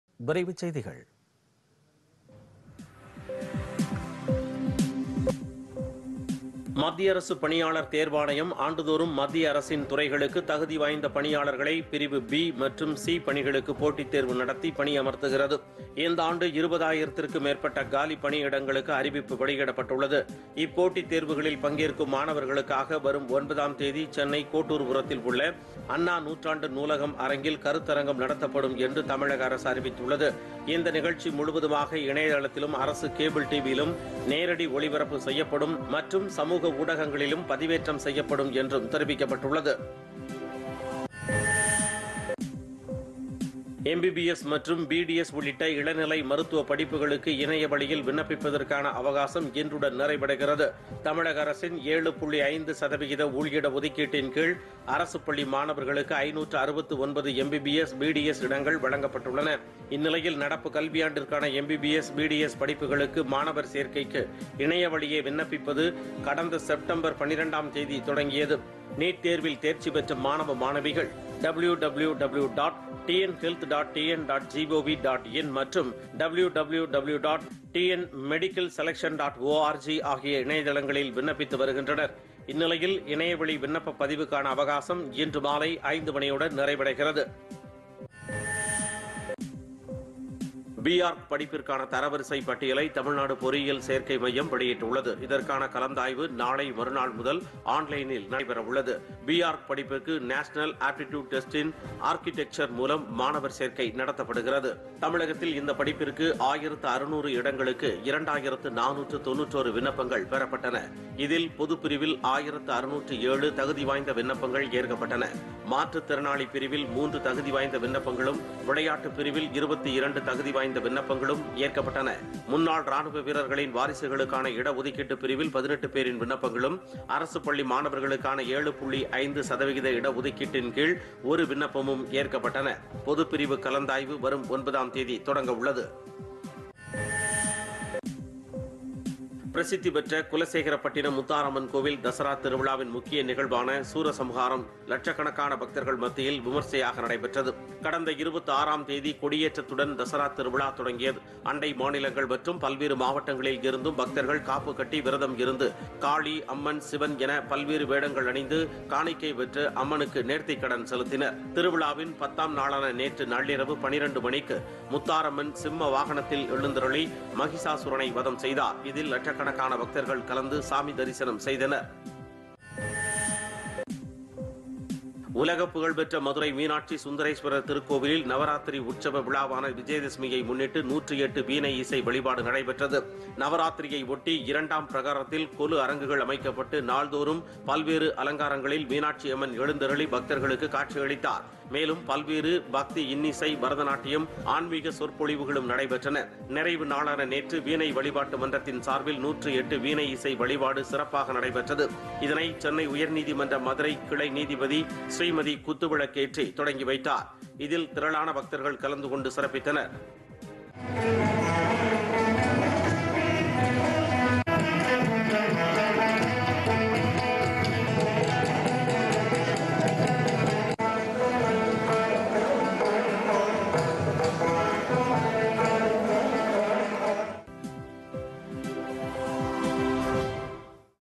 பொதிகை காலை 11.00 மணி விரைவுச் செய்திகள் [06.10.2022]PodhigaiTamilNews
1)  மாணவர்களுக்கு கருத்தரங்கம்

2) ஆன்லைன்  விண்ணப்பத்துக்கு இன்று கடைசி நாள்

3) பி.ஆர்க். படிப்பு - 8-ம் தேதி முதல் கலந்தாய்வு

4) விமரிசையாக ந‍டைபெற்ற சூரசம்ஹாரம்

5)108 வீணை இசை வழிபாடு